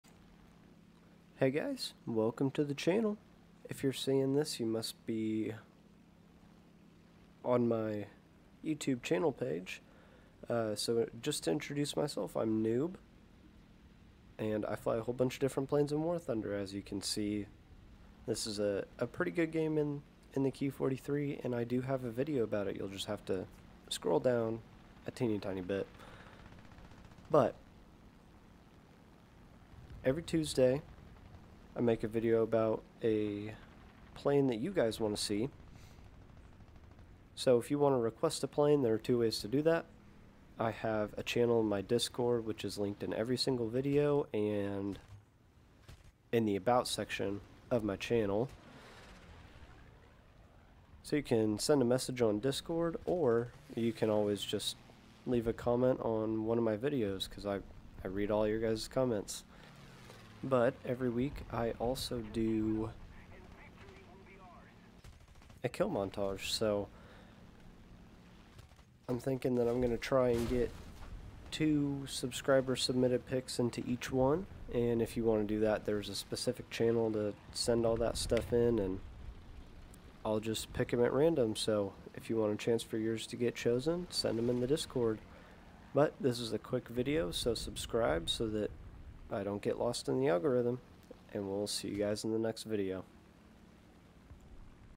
WELCOME TO THE CHANNEL
Just a short video telling you guys a little about what I do on her and how to submit planes requests or clips for Kill of The Week.